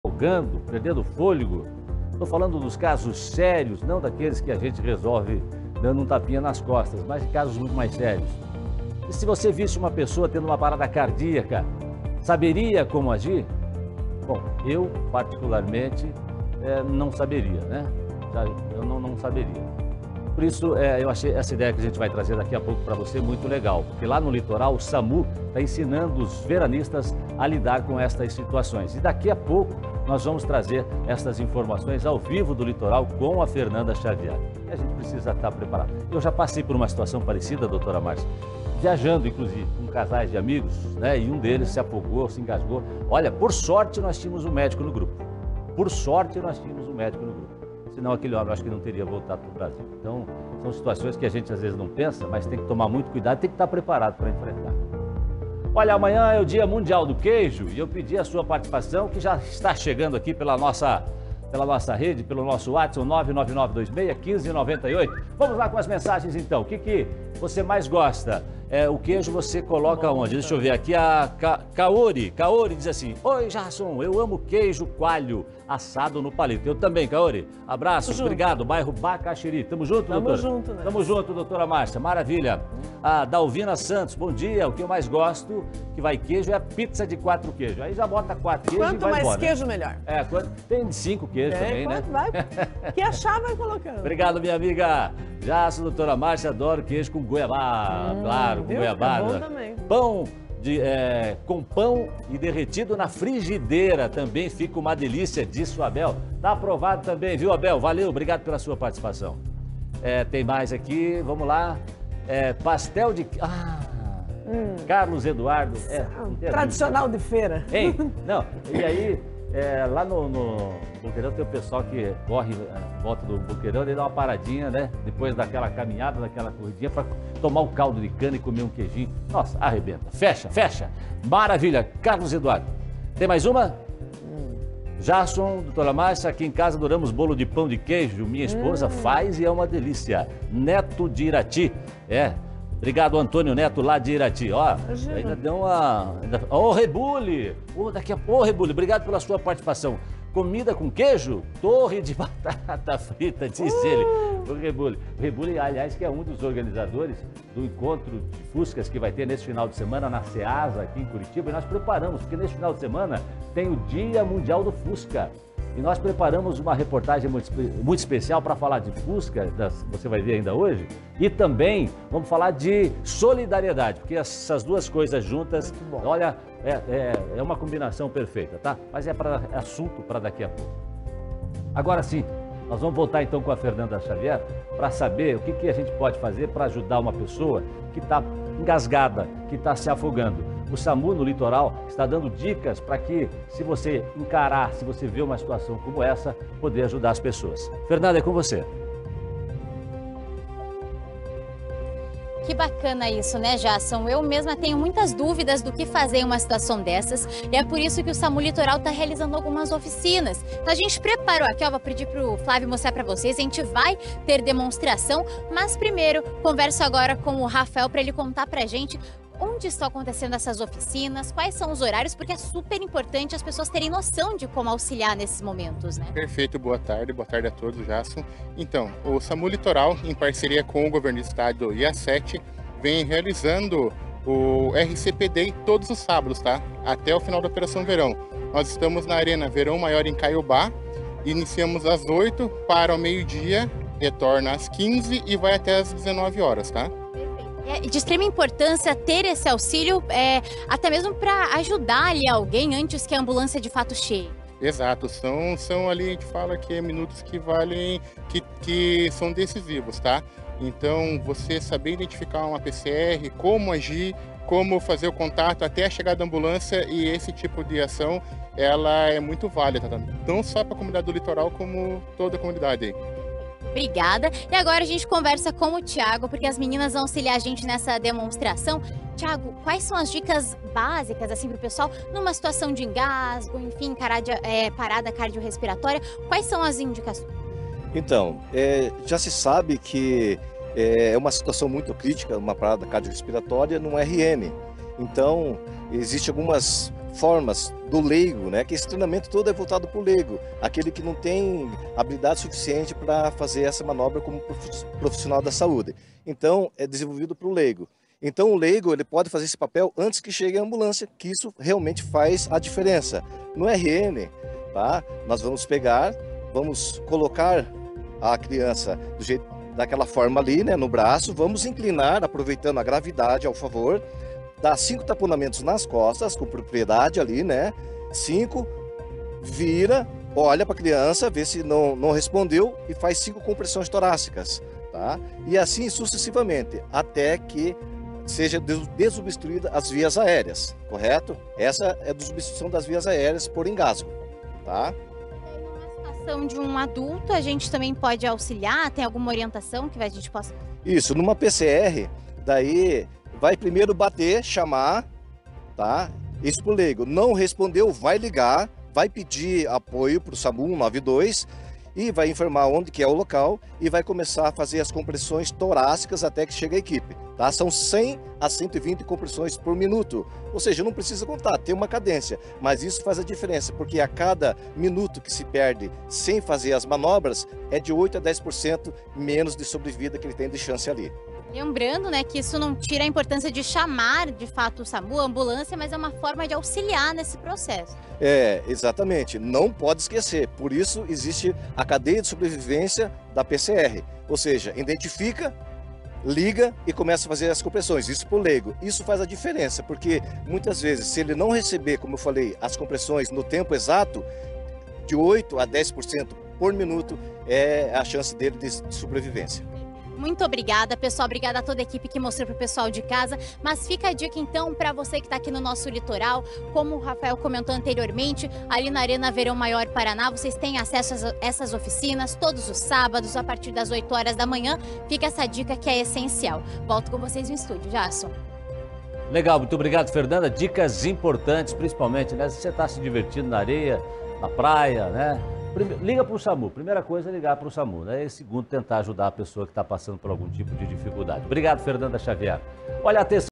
Você saberia como agir durante uma emergência; Samu ensina veranistas como atuar nessas situações
No litoral!